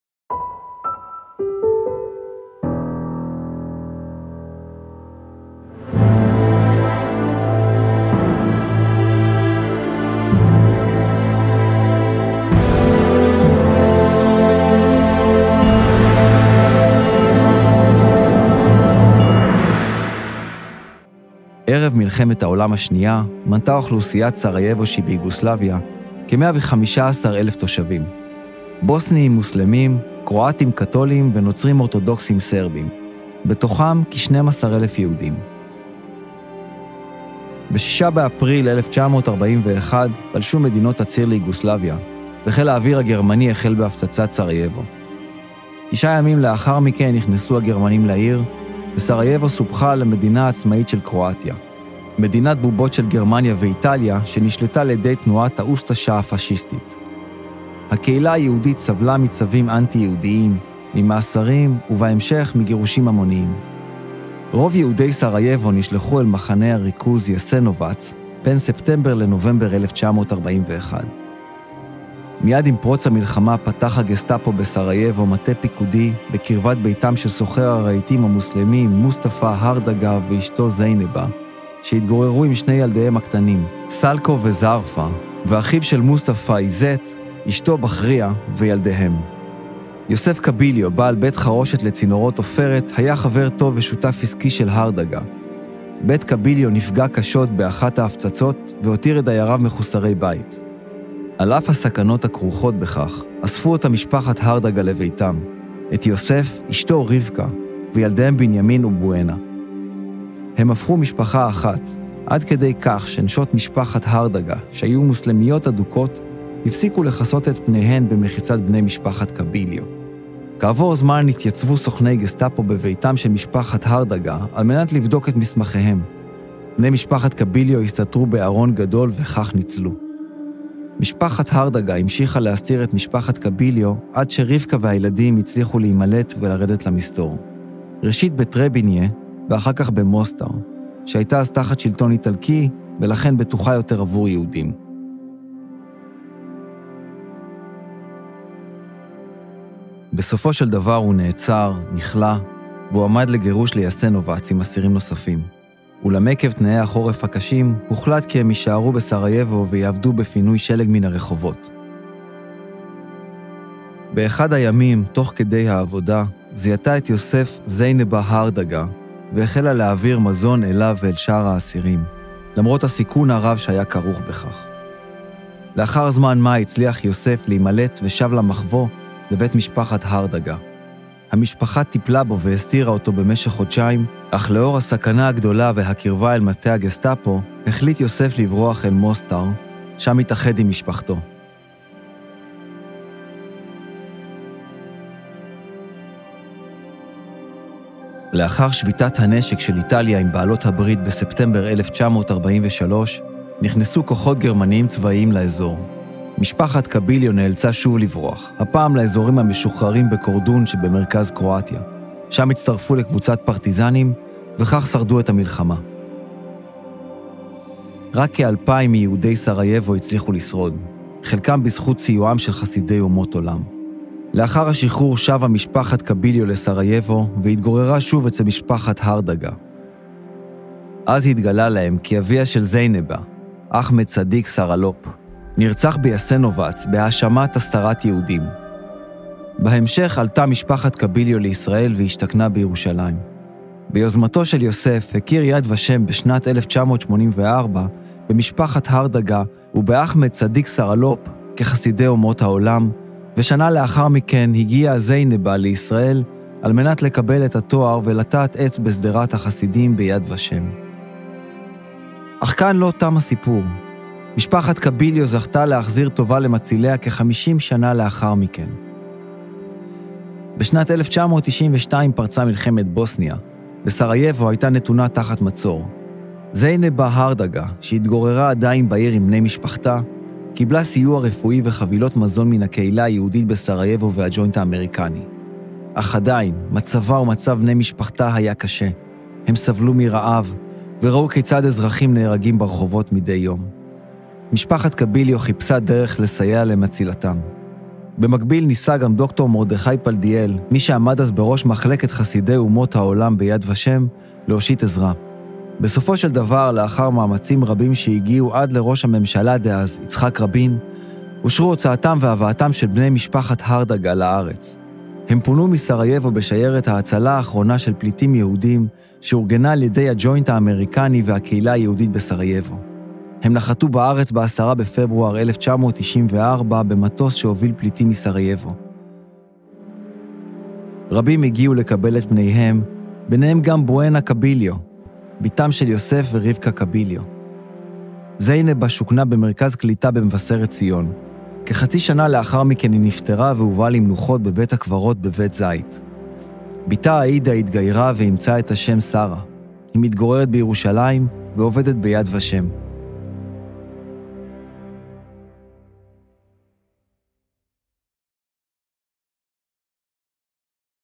סיפורה של זיינבה הרדגה | חסידי אומות העולם | לומדים שואה עם יד ושם
סרטון היסטורי המתאר את חייה של חסידת אומות העולם זיינבה הרדגה. הרדגה הצילה את משפחת קביליו בבוסניה שביוגוסלביה. הסרטון מלווה בקריינות ותמונות ארכיון. לסרטי דמויות נוספים